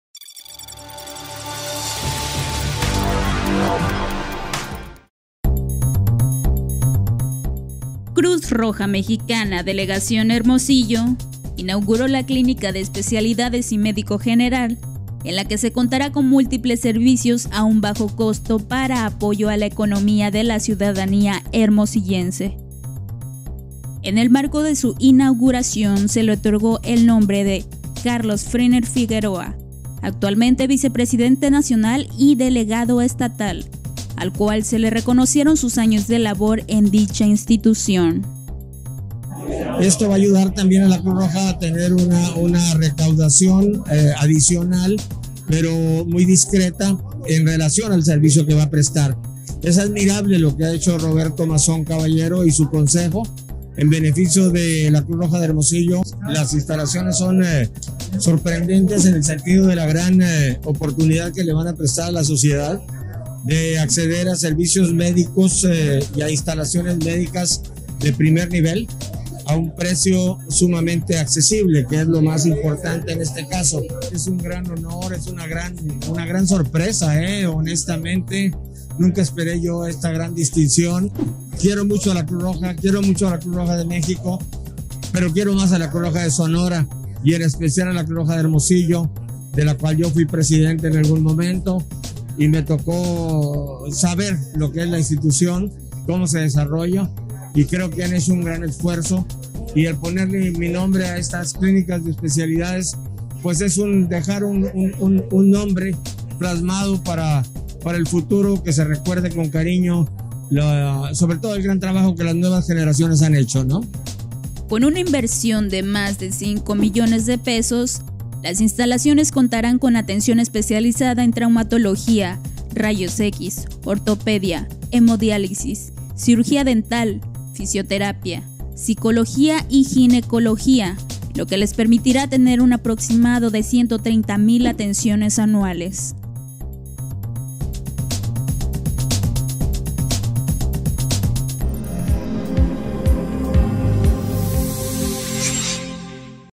Cruz Roja Hermosillo inaugura clínica de Especialidades y Médico General
La clínica de Especialidades y Médico General "Carlos Freaner Figueroa", cuenta con equipo para brindar múltiples servicios a costos accesibles, en apoyo a la economía de la ciudadanía hermosillense.

📹 Polet Ruano